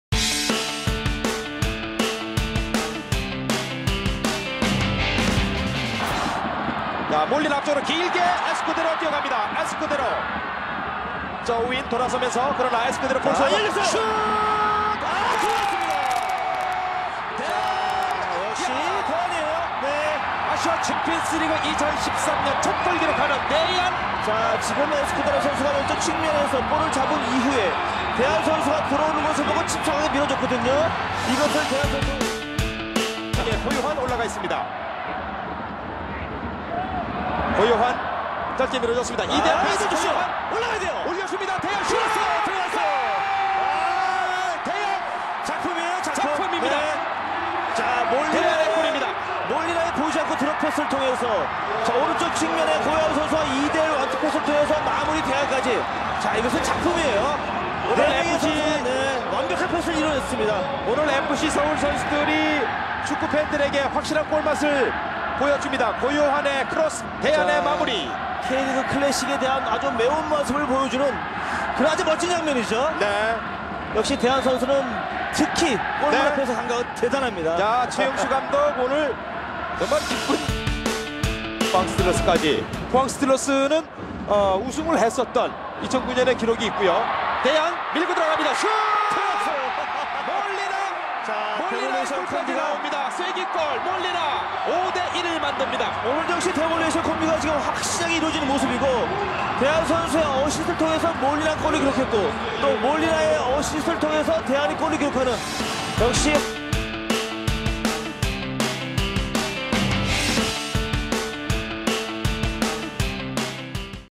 [2013 FC서울] #1. 패스의 정석
2013시즌 첫 경기로 ACL 1차전을 5-1 대승으로 시작한 FC서울!!

패스의 정석을 보여주며! 축구게임에서나 볼 수 있는!

기가막힌 킬패스와 그에 이은 골 영상 모음입니다!^^

FC서울의 패스의 정석을 보고 싶다면!

3월 2일 오후3시! 서울월드컵경기장! FC서울vs포항!